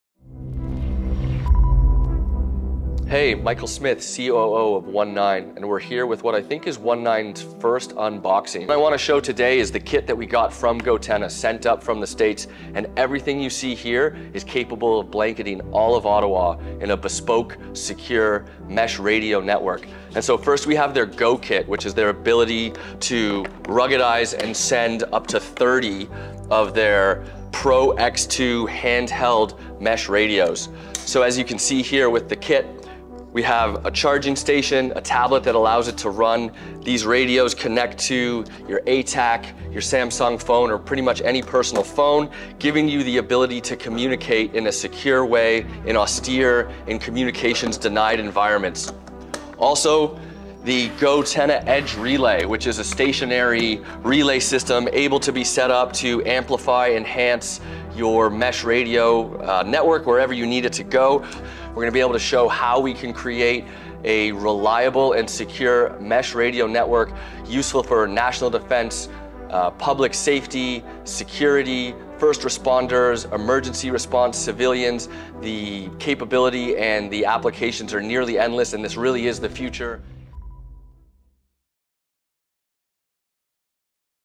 ONE9 Unboxes goTenna’s EdgeRelay and goKit 2
Michael Smith of Canadian venture firm ONE9 unboxes and looks at the goTenna Deployment Kit 2, aka goKit 2, and the new goTenna EdgeRelay - an  ad-hoc infrastructure support node that covers two goTenna mesh networks persistently.

ONE9, Canada’s first and only dual-use tech venture fund, officially became goTenna’s first international capital investor in 2023, focusing on expanding goTenna’s off-grid and easily deployable communications solutions to Canadian-specific use cases and end users.

Follow our Instagram:   / gotennapro
Follow our Twitter:   / gotennapro
Follow our Facebook:   / gotennapro
Follow our LinkedIn:   / gotenna-pro